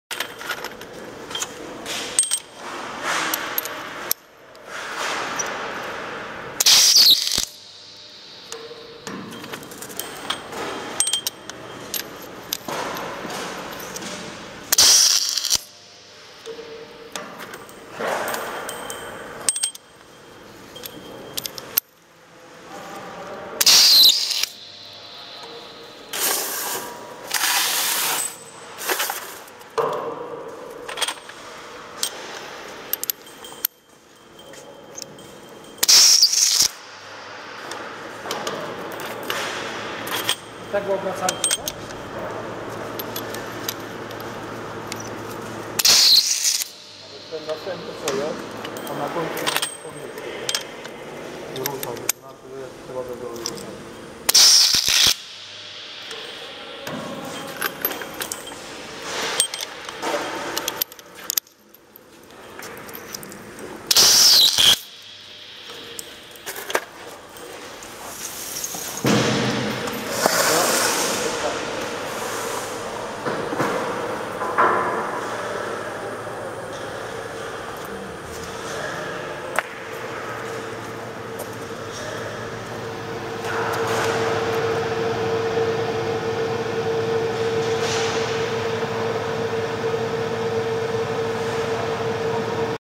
Zgrzewanie trzpieni 19 mm do belek mostowych.
Zgrzewarka  Nelson Intra 1400 / 2100 ,pistolet P-NS 20 BHD
Zgrzewanie trzpieni Nelson (19 mm) do belek mostowych.